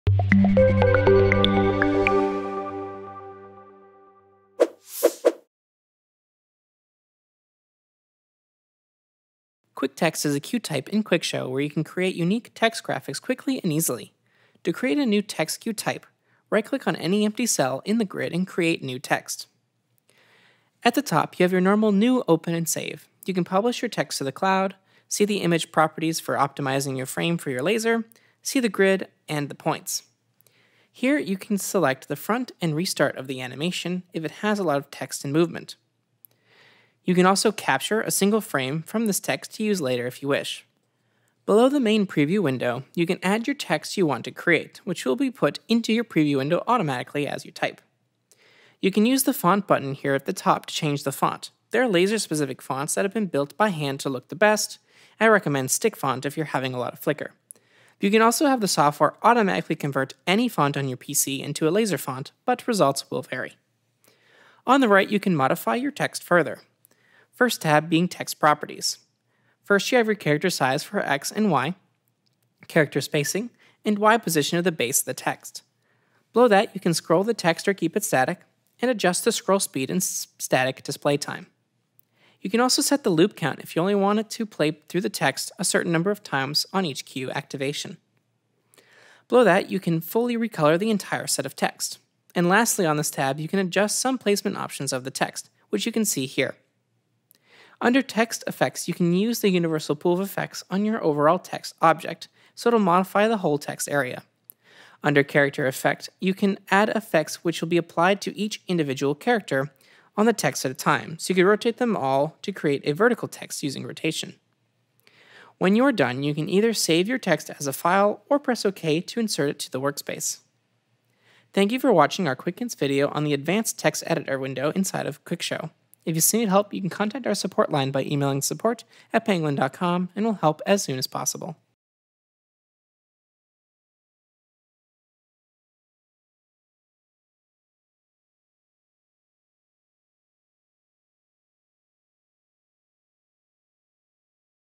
How to Use the Advanced Text Editor in QuickShow
In this video we discuss how to use the Advanced Text Editor to create text for your lasers.

Chapters
0:00 Introduction
0:20 Window Overview
0:49 Fonts
1:04 Text Modifiers
1:37 Text Effects
1:55 Saving your Text
2:05 Outro

We hope you enjoy learning everything you have wanted to learn about Quickshow in these series of videos!